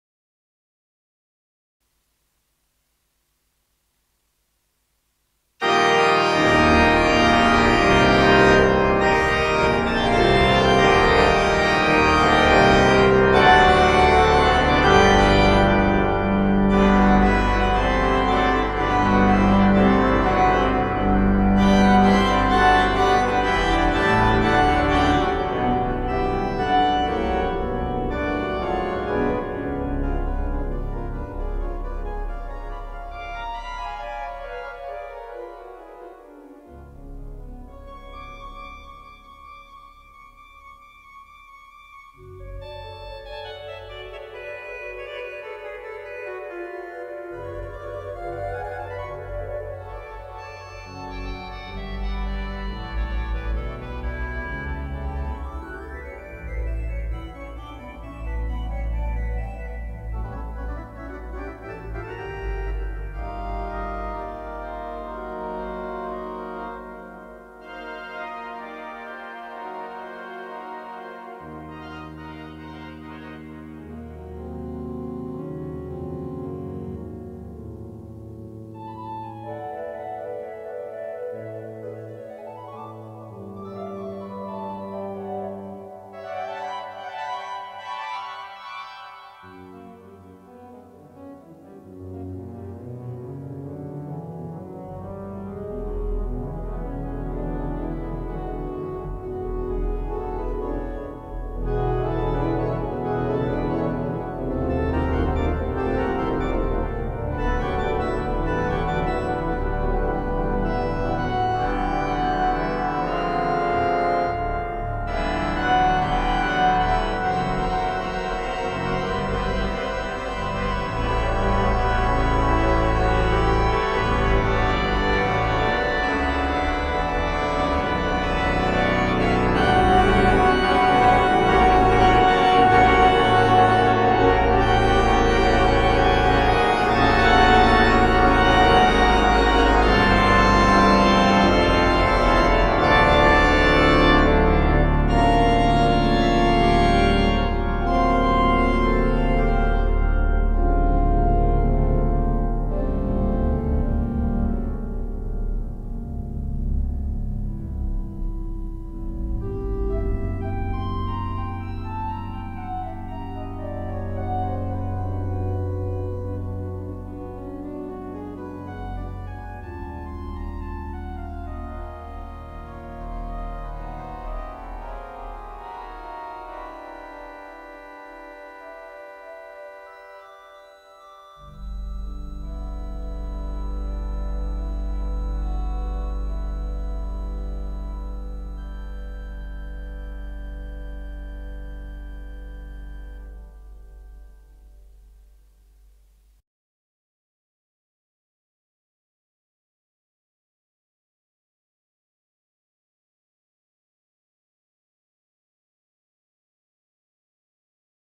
Pipe organ Improvisation on Aeolus with custom voicings
This video is a demonstration of how Aeolus pipe organ emulator can sound when all stops are voiced together as a complete organ.
All stops of this organ are voiced by Kerry Beaumont, UK organist and composer. He also played this improvisation where you it goes through 31 combinations with the organ.